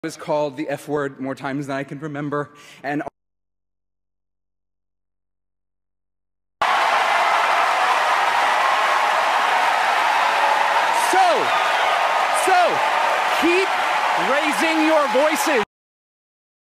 Michael Arden’s bleeped-out Tony Award 2023 speech is instantly iconic
Growing up, I was called the F-word more times than I could remember. And all I can say now is I'm a faggot with a Tony!